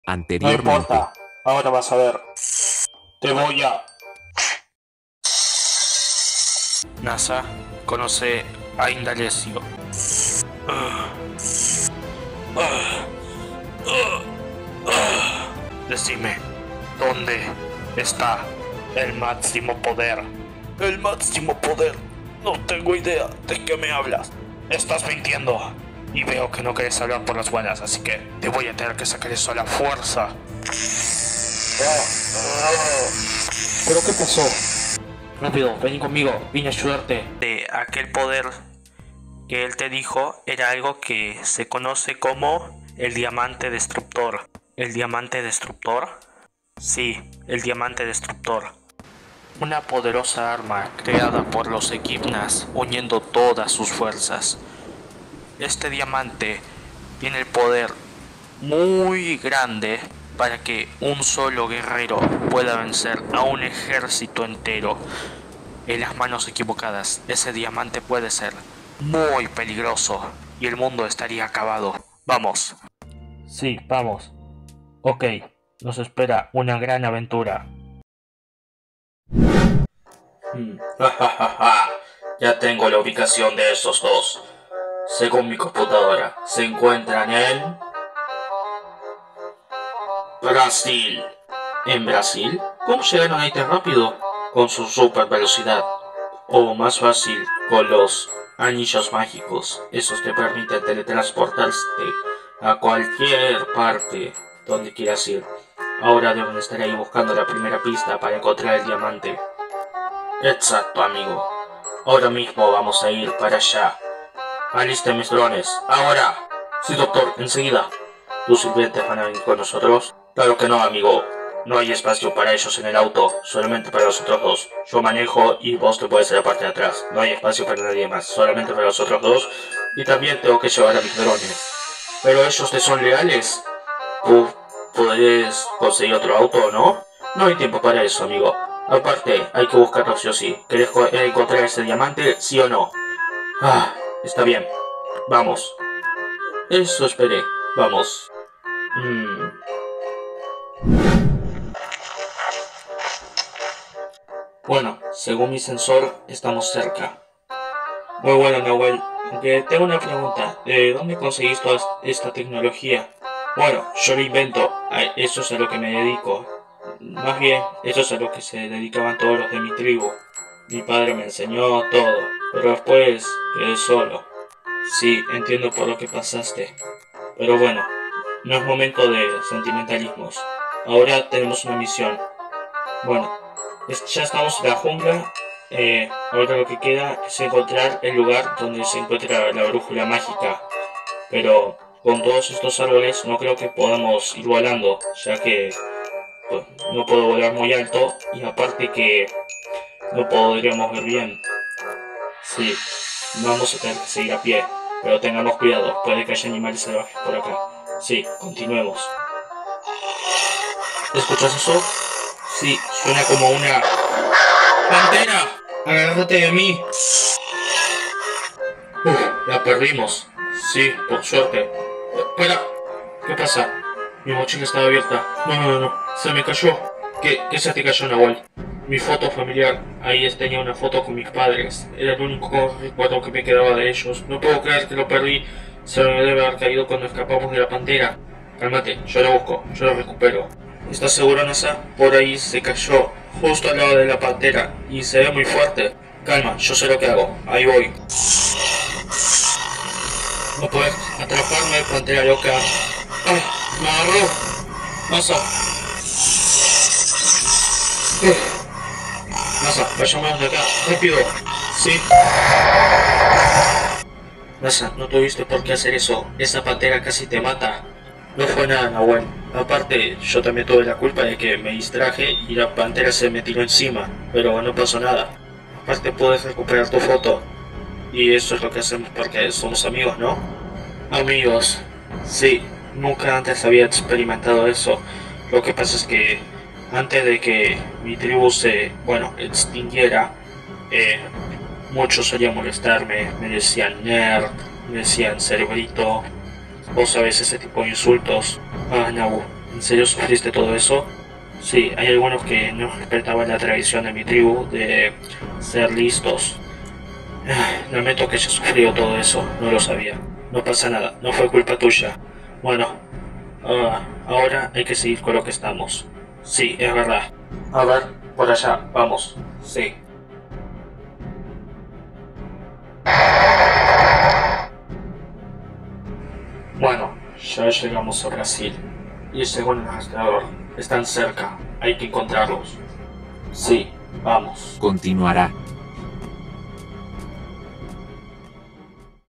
SONIC LEGO CINEMA: La Jungla Amazónica
Después de conocer a Nahuel, nos dirigimos a la Jungla Amazónica, donde empezamos a buscar la primera pista para encontrar el diamante. Sin saber lo que nos esperaba ahí.